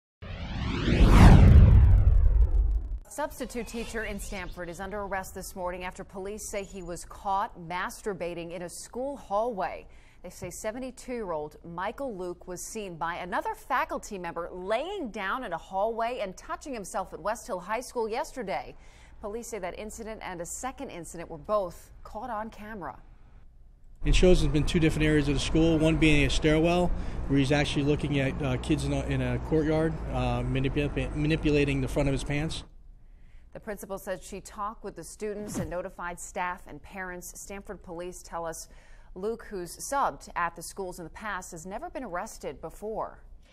Teacher Caught Masturbating In School Hallway
Police say a substitute teacher at a Connecticut high school was caught masturbating Wednesday in a school hallway as he watched students in the courtyard, The Hartford Courant reported.

Michael Luecke, 72, was arrested for public indecency, breach of the peace and risk of injury to a minor, according to reports.

A faculty worker at Westhill High School in Stamford told police she spotted Luecke lying on the ground and thought he was injured. But on closer inspection, she saw him "manipulating his penis," police said, per the Courant.

Video surveillance footage also shows him in a second suspiciously lewd act, Stamford Police Lt. Diedrich Hohn told News 12 in the segment above.

"The principal took immediate action and called the police," Stamford Public Schools rep Sharon Beadle told the New York Daily News. Beadle said Luecke had worked at the school since 2009.

Hohn told the Daily News that Luecke had no criminal record.

Luecke was filling in for a music teacher, News 12 mentioned.

He was placed on suicide watch after the incident and was held on $25,000 bond, the Stamford Advocate reported.

Credit - Ron Dicker